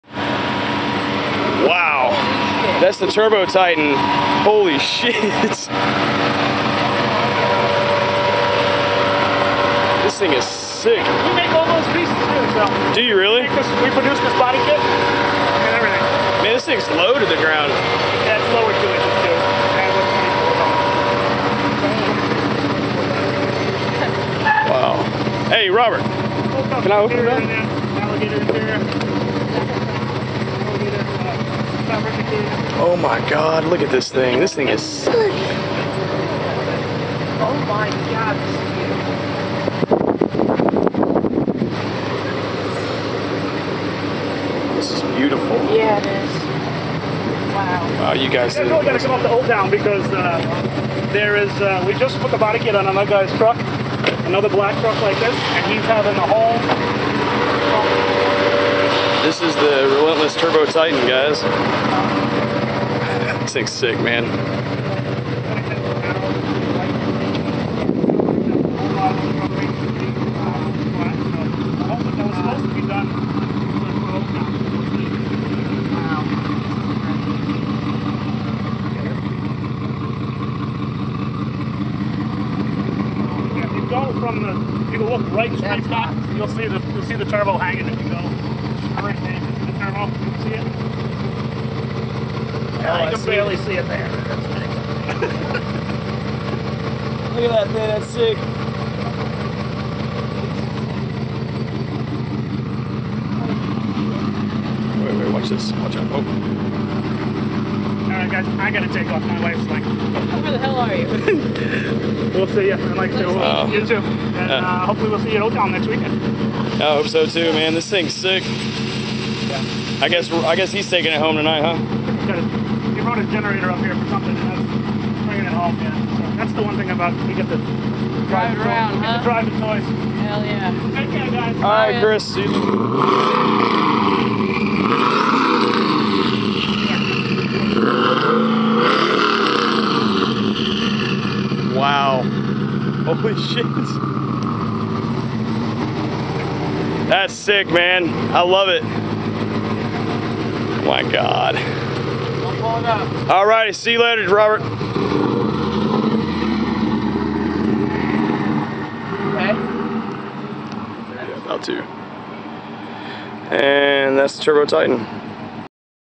RELENTLESS RPM TURBO TITAN
Relentless RPM Turbo Titan, Walkaround with idle and REVS! View of turbo mounting location.

This Turbo Titan, hands down is sick! Relentless RPM does great work and stand behind all their quality and customer service. Top notch staff and amazing craftsmanship. They will continue to get my business!